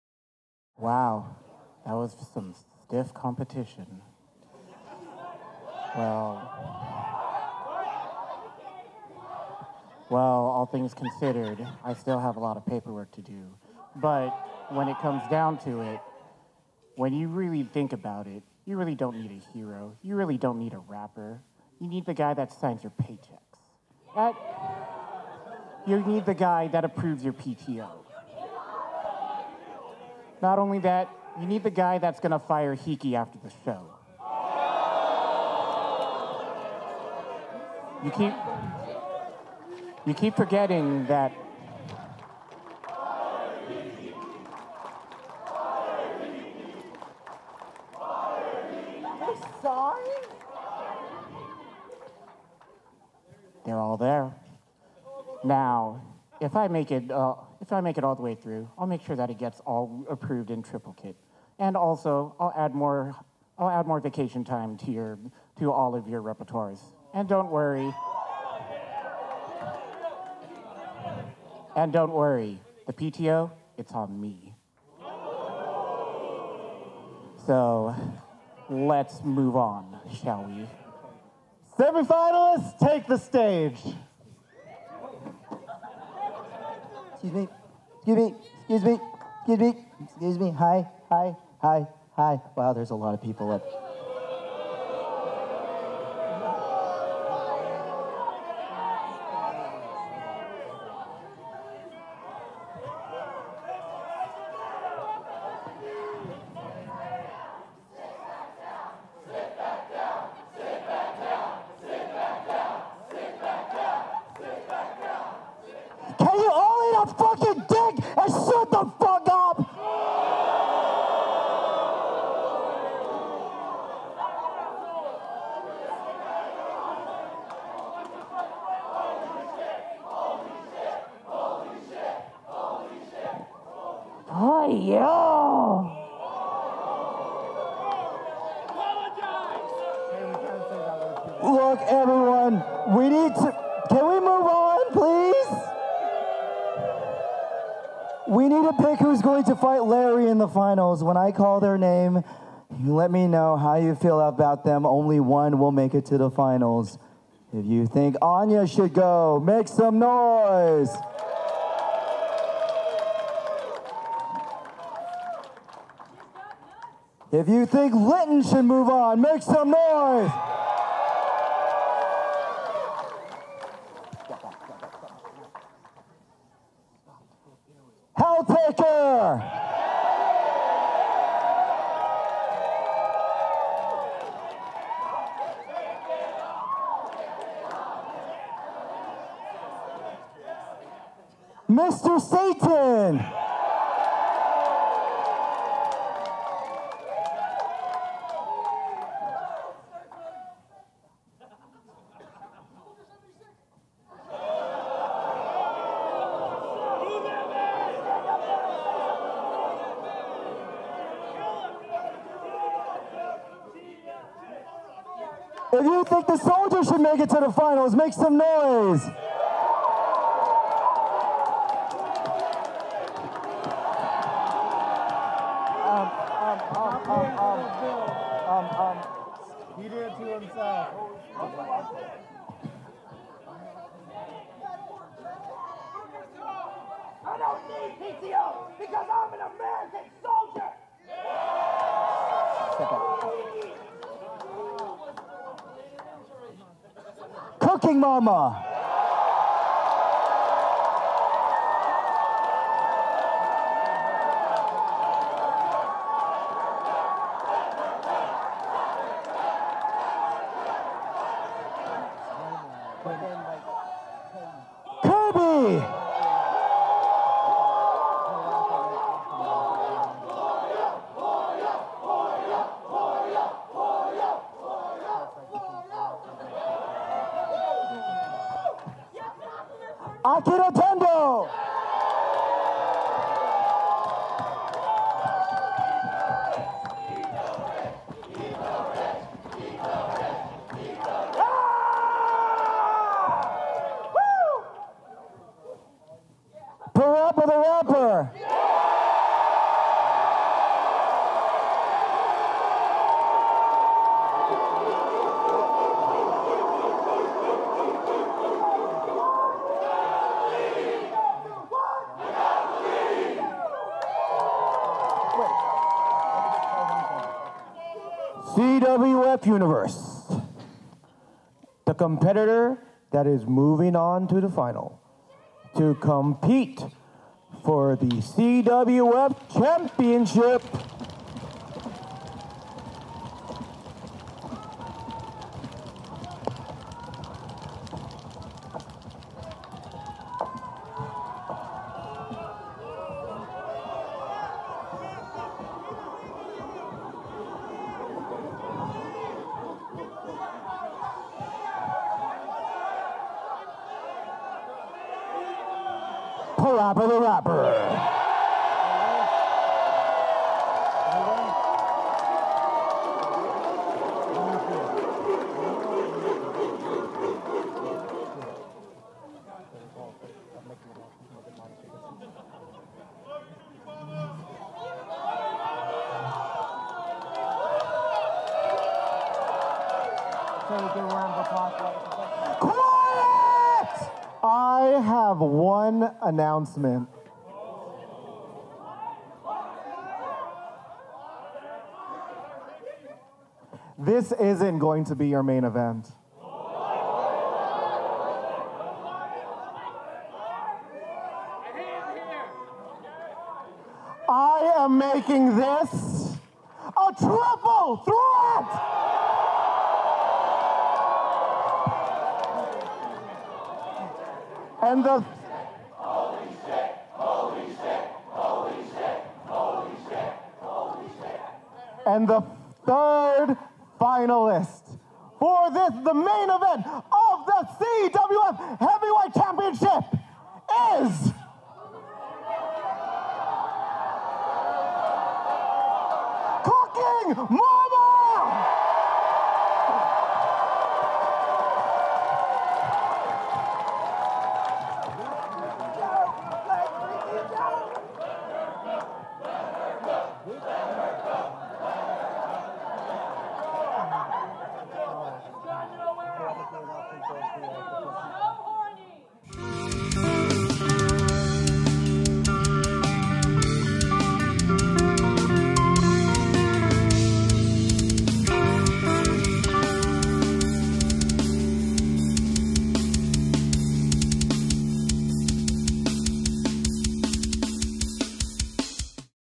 FanimeCon 2024 | Event - CWF: Fanimania 9 - Larry Response & Final
The CWF celebrates 10yrs of operating, and this night is Fanimania 9! I guess Larry gets to respond. Then Cooking Mama gets the 2nd round started.  @CosplayWrestlingFederation
Related Links
Cosplay Wresting Federation